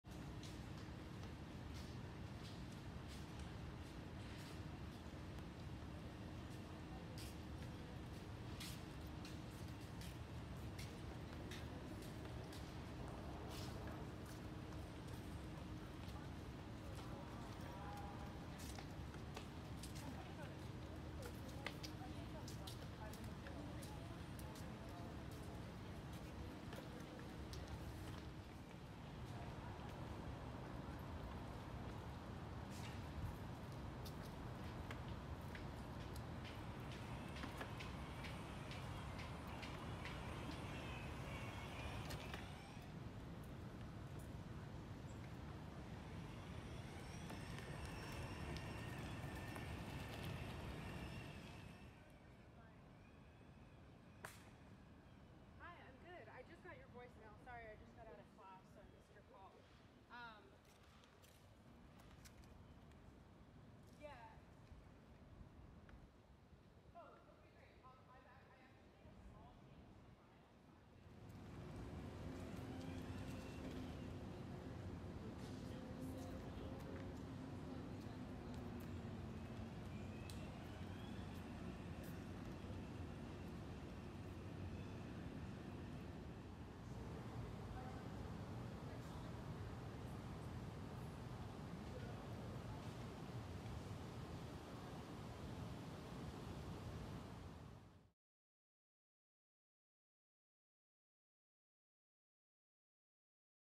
UCLA - Scenery #18 (Food Delivery Robot)
It’s just a simple video of casual daily scenery. Ordinary things around us give us new insights.